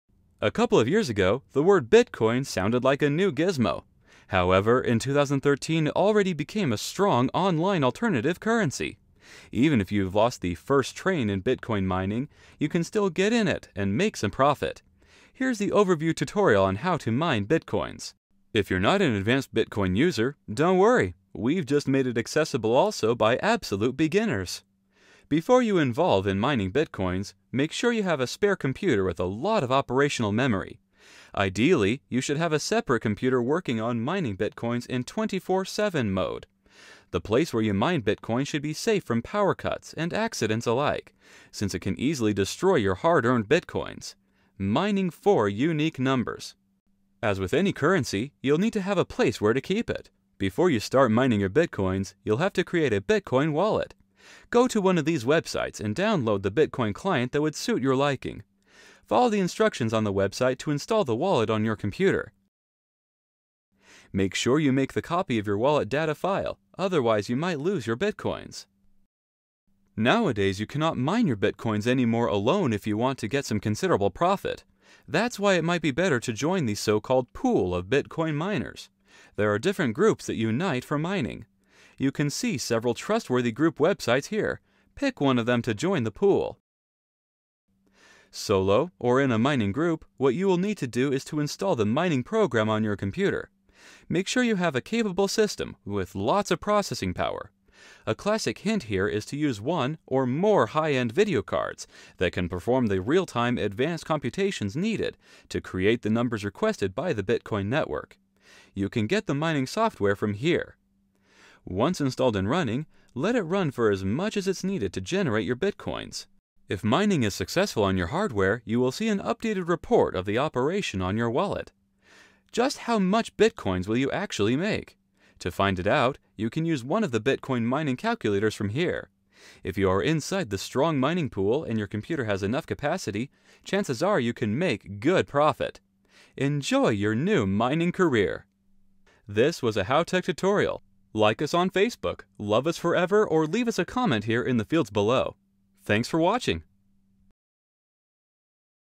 How to Mine Bitcoins
A couple of years ago, the word “Bitcoin” sounded like a new gizmo. However, in 2013 it already became a strong online alternative currency. Even if you’ve lost the “first train” in Bitcoin mining, you can still get in it and make some profit. Here’s the overview tutorial on how to mine Bitcoins. If you’re not an advanced Bitcoin user - don’t worry. We’ve just made it accessible also by absolute beginners.

Step 1.  Before you involve in mining Bitcoins, make sure you have a spare computer with a lot of operational memory. Ideally you should have a separate computer working on mining Bitcoins in 24/7 mode. The place where you mine Bitcoins should be safe from power cuts and accidents alike, since it can easily destroy your hard earned money.

Step 2. As with any currency, you’ll need to have a place where to keep it. Before you start mining your Bitcoins, you’ll have to create a Bitcoin wallet. Go to the link you see on screen ) and download the Bitcoin client that would suit your liking. Follow the instructions on the website to install the wallet on your computer. Make sure you make the copy of your wallet data file, otherwise you might lose your Bitcoins.

Step 3. Nowadays you cannot mine your Bitcoins anymore alone if you want to get some considerable profit. That’s why it might be better to join the so-called “pool” of Bitcoin miners. There are different groups that unite for mining. You can see several trustworthy group websites
 - pick one of them to join the pool.

Step 4.  Whether alone or in a mining group, what you will need to do is to install the mining program on your computer. Make sure you have a capable system, with lots of processing power. A classic hint here is to use one, or more high-end video cards that can perform the real-time advanced computations needed to create the numbers requested by the Bitcoin network.

Step 5.  Once installed and running, let it run for as much as it's needed to generate your Bitcoins. If mining is successful on your hardware, you will see an updated report of the operation on your wallet.

Step 6.  Just how much Bitcoins you will actually make? To find it out, you can use one of the Bitcoin mining calculators from the link you see on screen you are inside the strong mining pool and your computer has enough capacity, chances are you can make good profit.

Result: Enjoy your new money-mining career!